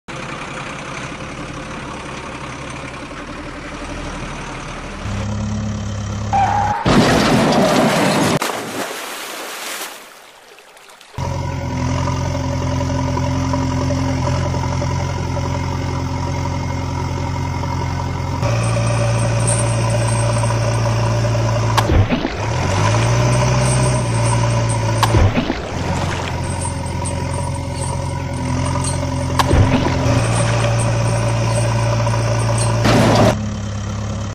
JCB 3DX And MAN Truck Accident Big River Pulling Out JCB 5CX Plus Loader | CS Toy Shorts
CHETAN Bhai  And Bhai CHUMMAN

CS Short  Team :- Anuj Bhai Ritesh Bhai Mahend Lekhram Bhai Vikesh Netram Bhai

Titles :-  JCB 3DX And MAN Truck Accident  Big River Pulling Out JCB 5CX Plus Loader | CS Toy Shorts